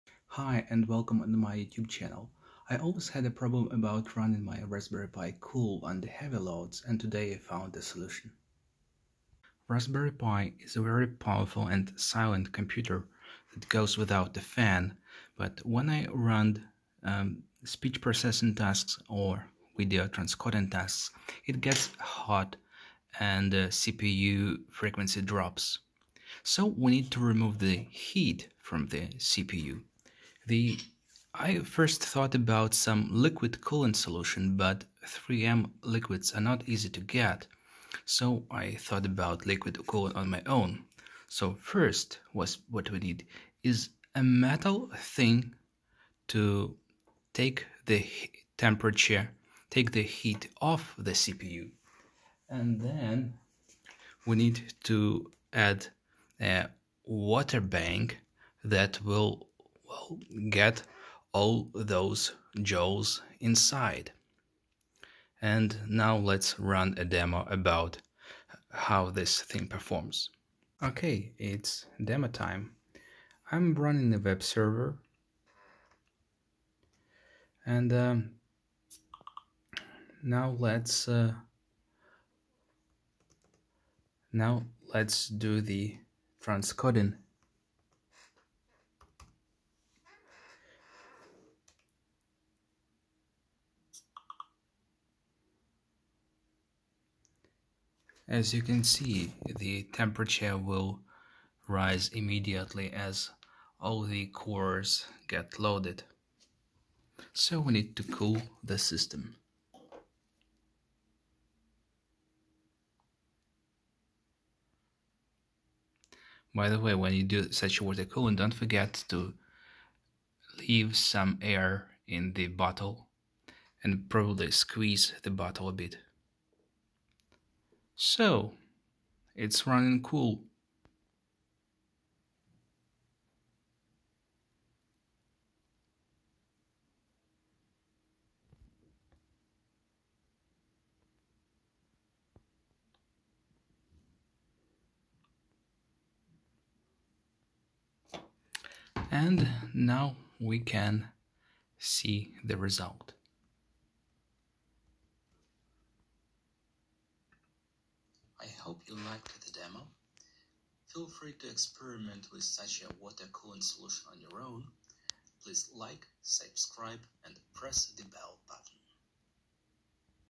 Raspberry Pi simple water cooling solution
I've came up with an idea of keeping raspberry pi cool (temporarily) under heavy loads.